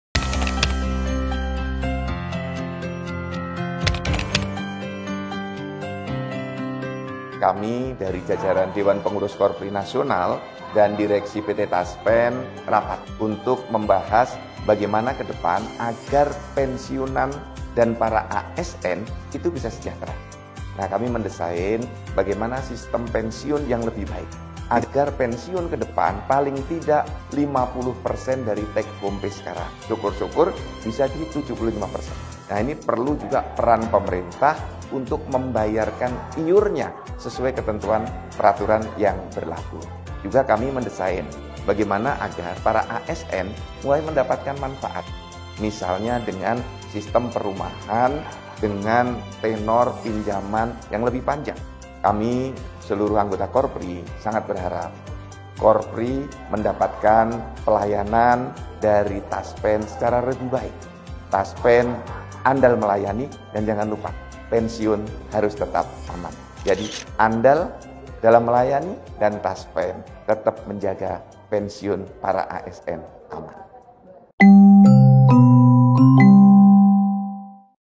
KORPRI dan TASPEN Bersinergi Tingkatkan Kesejahteraan ASN! 🇮🇩
KORPRI dan TASPEN menjalin kerjasama strategis untuk meningkatkan kesejahteraan Aparatur Sipil Negara (ASN) dan pensiunan di Indonesia. Kolaborasi ini merupakan komitmen bersama untuk memastikan masa depan yang lebih cerah bagi para abdi negara dan keluarganya.

Fokus utama dalam sinergi ini adalah untuk menerapkan:
* Sistem Pensiun yang Lebih Adil dan Berkualitas
* Pelayanan, Fasilitas, dan Kemudahan Optimal
* Peningkatan Nilai Ekonomi dan Sosial

Ini juga merupakan komitmen TASPEN kepada ASN dan Pensiunan untuk senantiasa memberikan layanan terbaik dan menjaga kepercayaan serta keamanan dalam mengelola dana pensiun.

Mari bersama-sama dukung KORPRI dan TASPEN dalam mewujudkan kesejahteraan ASN dan pensiunan di Indonesia!